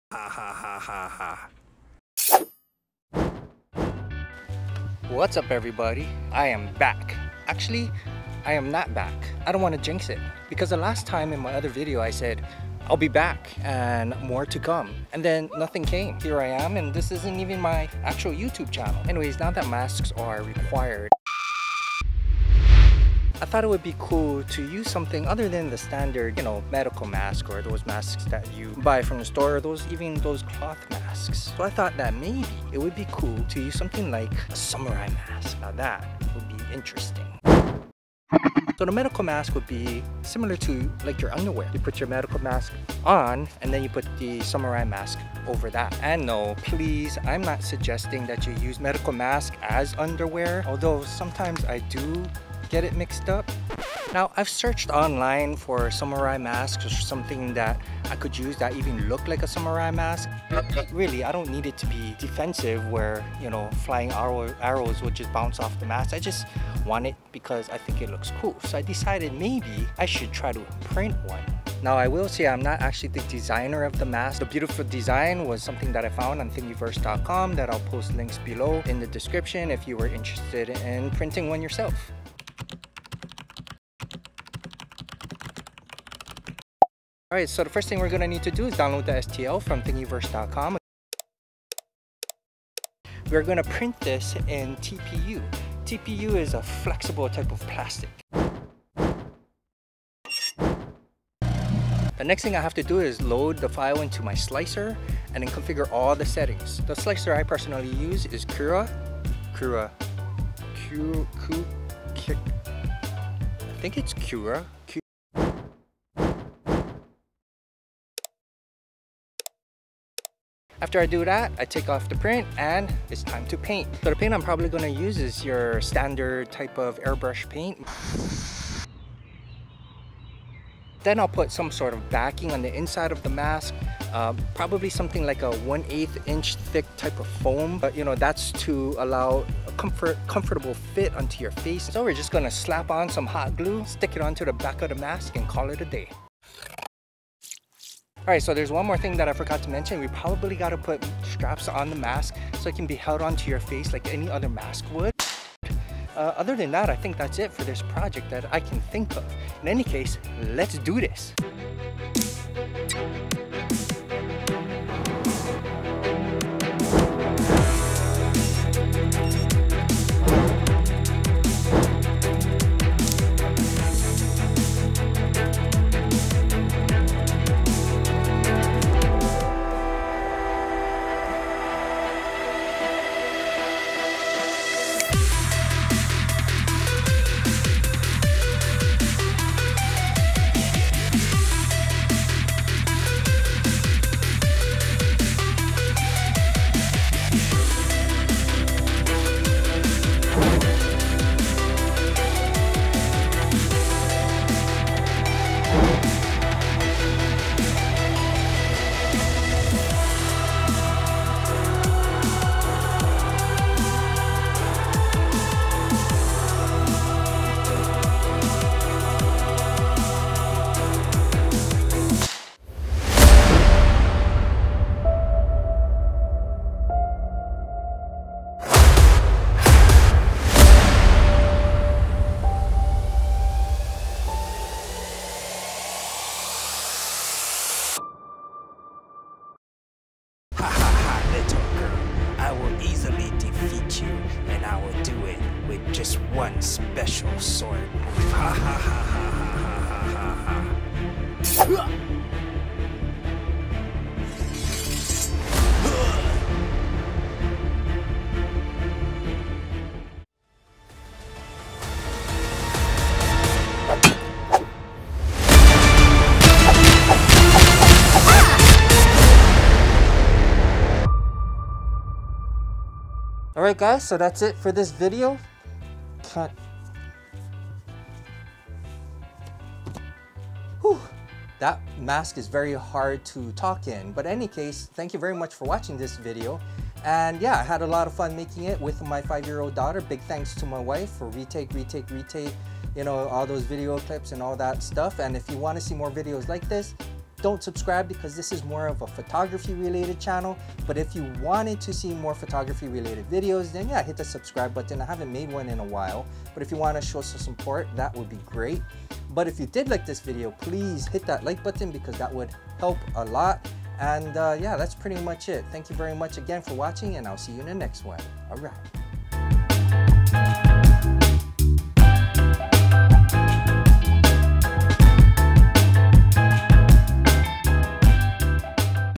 (Not A Photography Video) Quick How To: 3D printed Samurai Mempo Mask - Ender3 Pro - TPU
This was a fun video to make because it has been awhile since I've made a video due to medical challenges. Hopefully I can get back into it. Just trying to find the drive again. I needed to create something to re-spark the drive. Nothing to do with photography but it was still fun to do.

Youtube: The child in this video is my daughter and I consent to her being in this video.

STL download
Copyright of Design/3D model: Mask Design copyright to creator.
Designer by: flioink

Amazon store to gear I use.